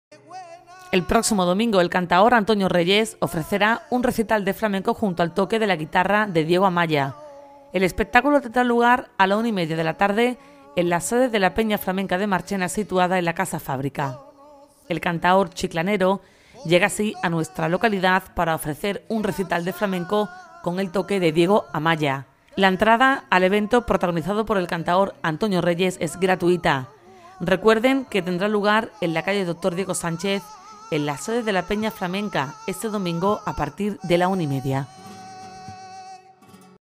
Este domingo Recital de Cante Flamenco en la Peña de Marchena
El próximo domingo el cantaor Antonio Reyes ofrecerá un recital de flamenco junto al toque de la guitarra de Diego Amaya. El espectáculo tendrá lugar a las 13:30h en la sede de la Peña Flamenca de Marchena situada en la Casa Fábrica.